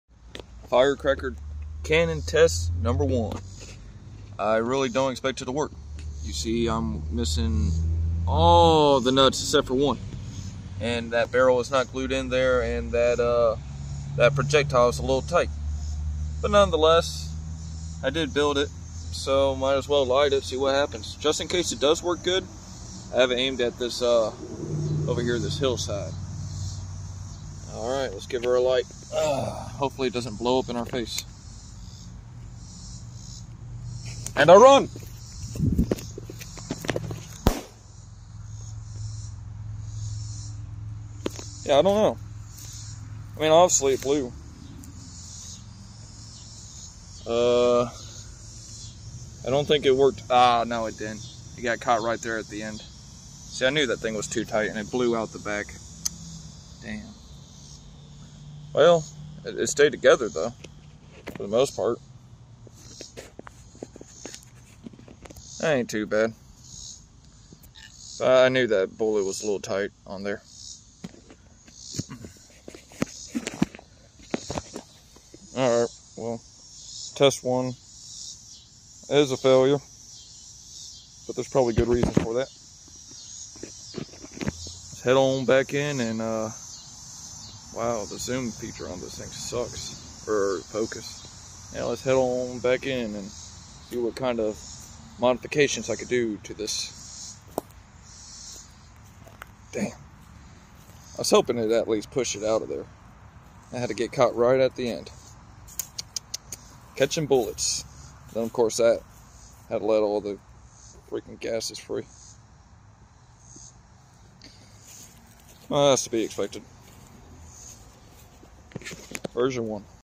firecracker 3d printed cannon fail, but it's only attempt one.
I knew the design had shortcomings, tried it anyways. always work to be done...